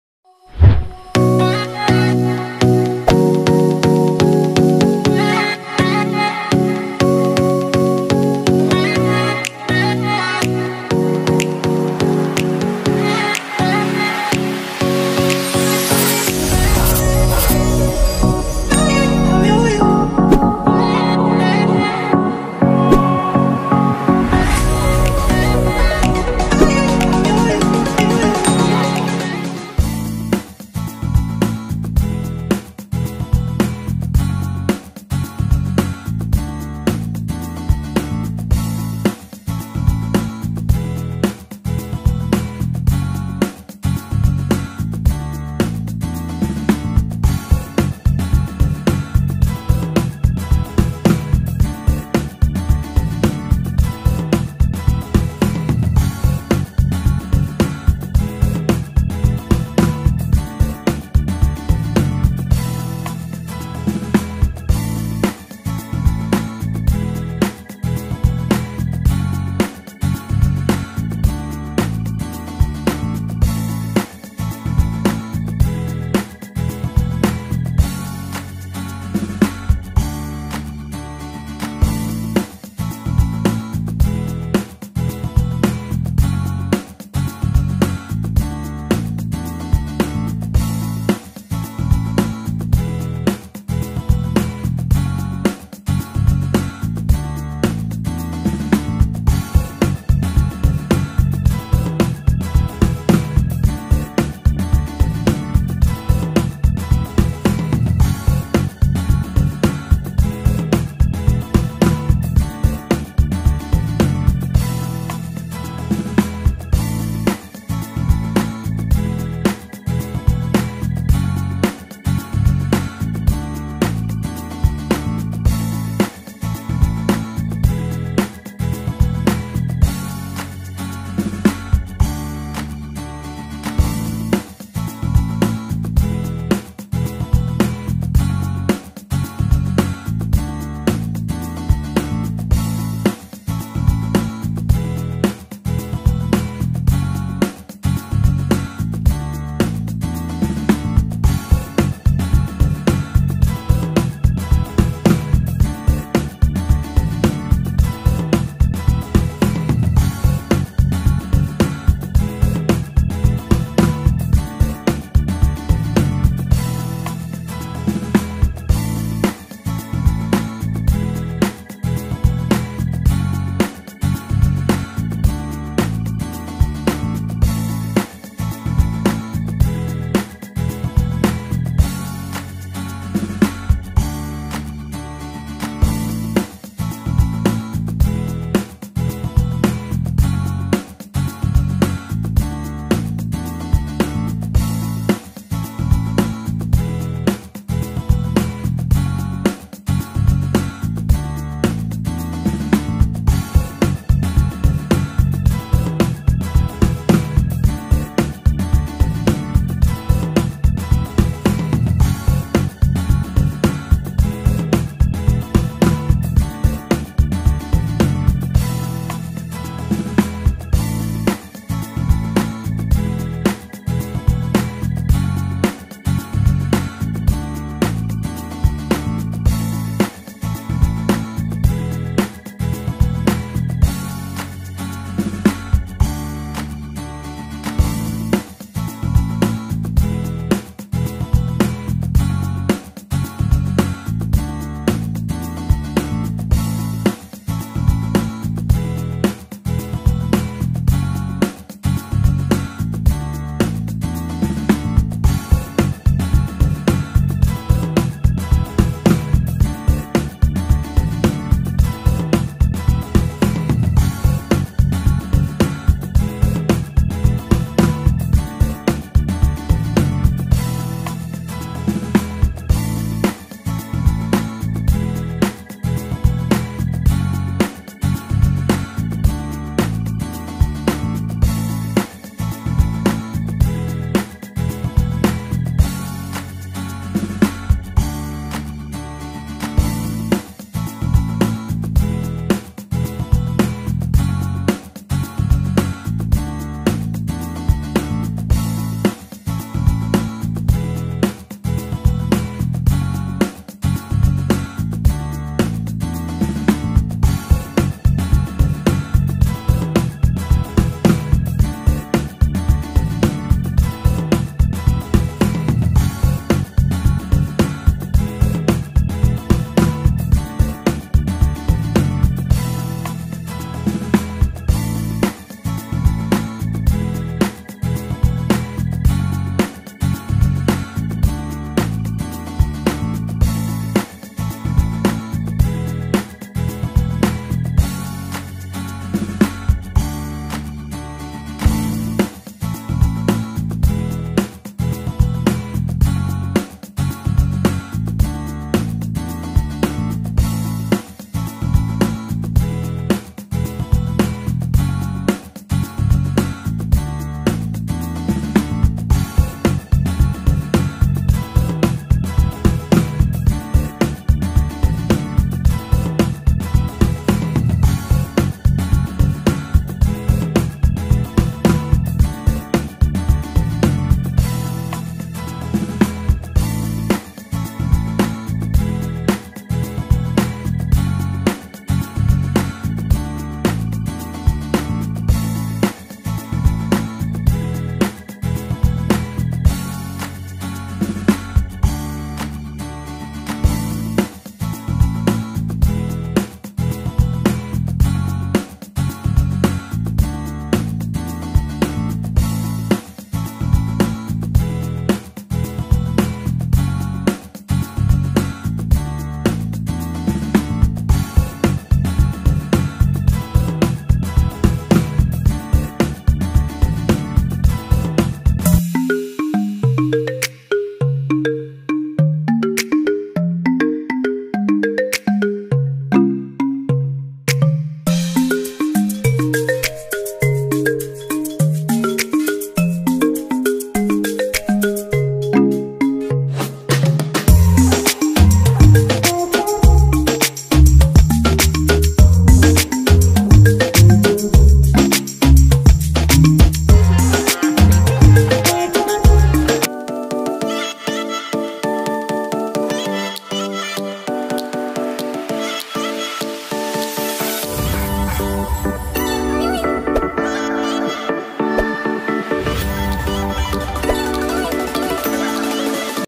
OILBULARYA SERIES: DIY PLANT SPRAY MADE WITH THHC + ESSENTIAL OILS 🍃🌱 | It's Me Dingot
Hello ka oilers!! Another Oilbularya series na naman handog ko para sa mga plantita/tito natin jan... introducing my DIY Plant Spray made with THHC plus a touch of Essential Oils!! Recipe down below!

In a 500ml Spray Bottle we will need a:
1 Capfull of Thieves Household Cleaner (THHC)
2-3 drops of Citrus Fresh Essential Oil Blend
1-2 drops of DiGize Essential Oil Blend
Fill the rest with Distilled Water